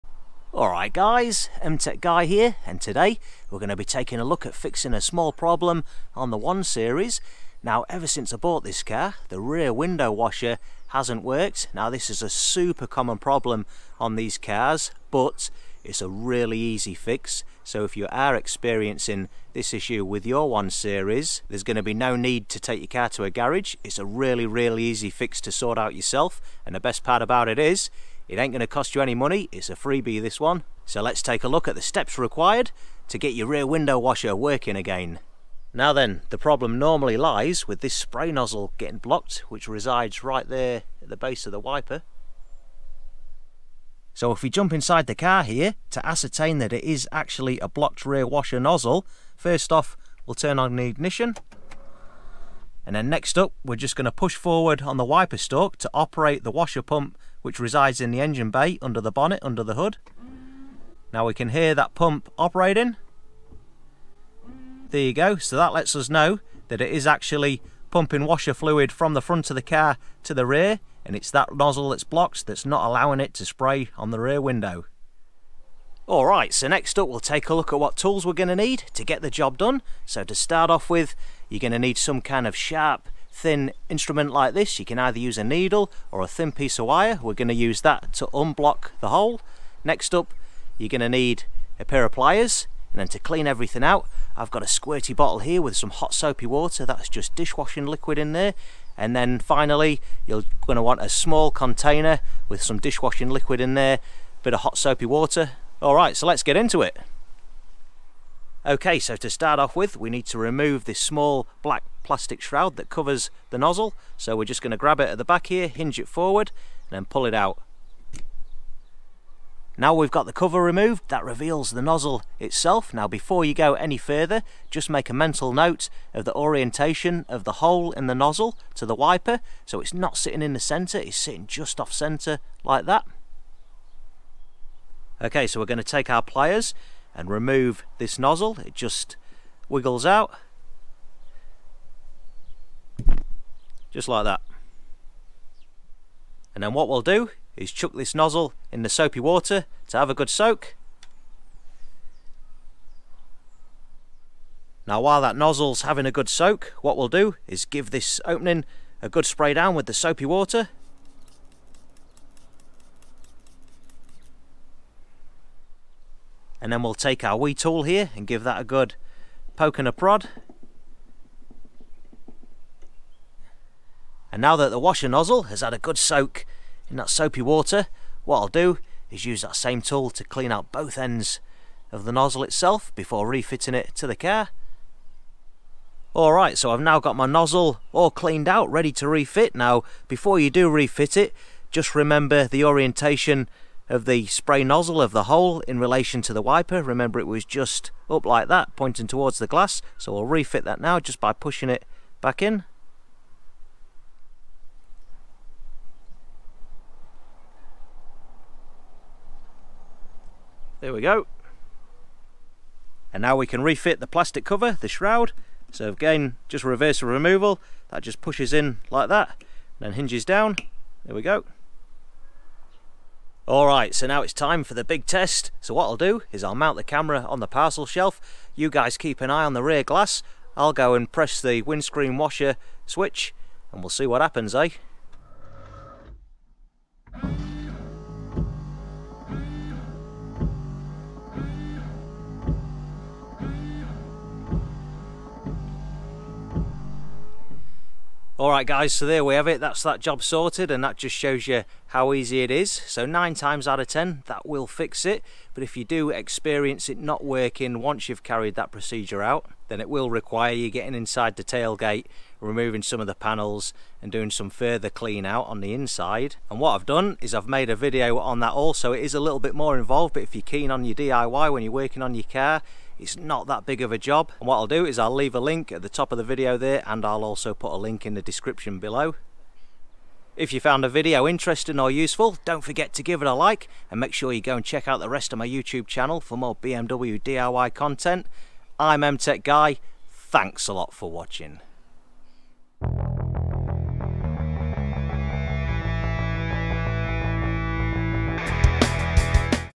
BMW 1 Series Rear Washer Fix
Let’s take a look at a very straight forward procedure to repair your rear window washer on your 1 Series BMW to get it working again.

9 times out of 10 this repair will solve the issue.
If it still fails to work, check out my second video where we take a more involved look at carrying out further repair internally at the tailgate to resolve the issue-